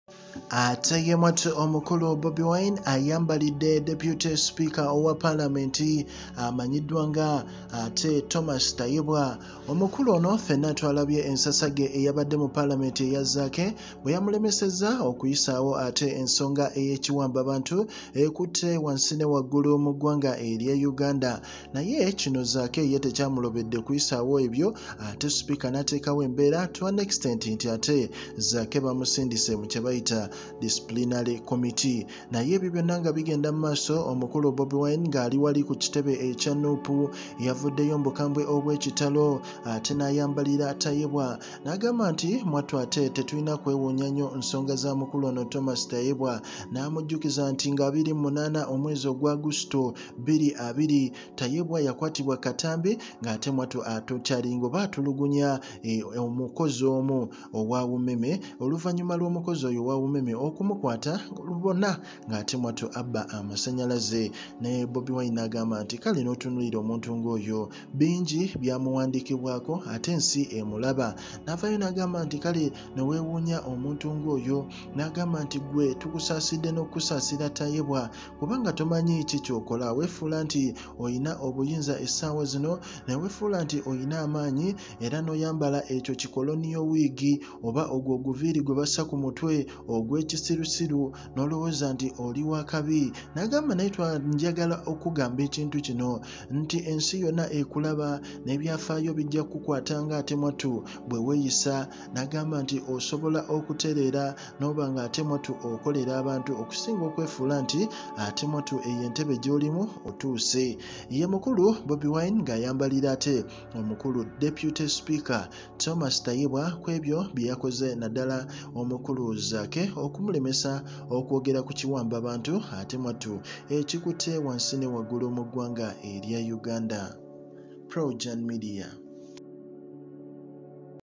Bobi wine Alayidde Okufa ne deputy Speaker tayebwa lwakukozesa bubi ofice ye nga Drone ziwawamba nup
Obusungu Museveni Alagide francis zaake atwalibwe mukakiko kempisa Lwakubanja ba nup drone bezatwala
kika Bobi wine kyeyateka mu Francis zaake kikambwe speaker wa museveni amujeeko akazindalo ngatabuse
Ono Bobi wine bajamuwulira Ekituuse Ku parliament ya uganda zaake atabuse lwakuwamba ba nup gabawede